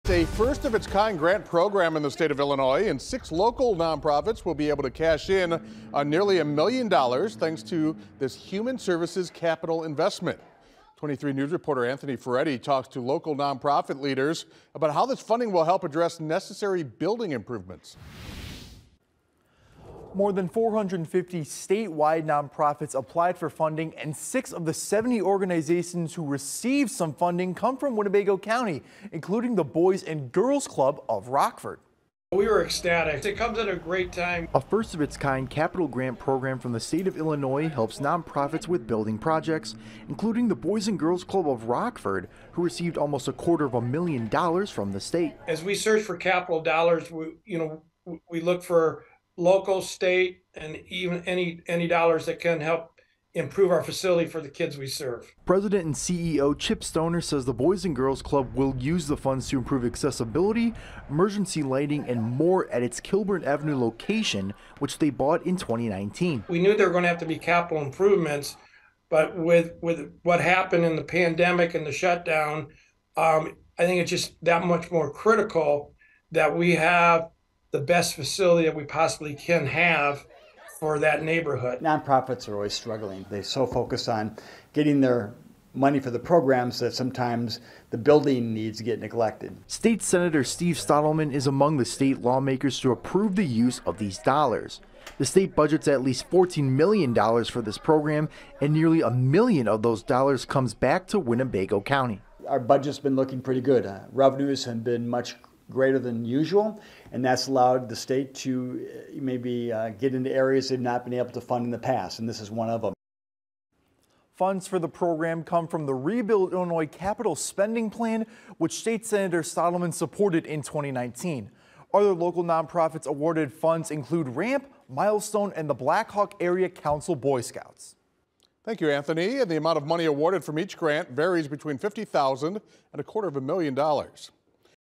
Nearly $1 million total distributed to area non-profits
It’s a first of its kind in the state of Illinois and six local non-profits will be able to cash in on nearly $1 million total, thanks to the Human Services Capital Investment Program.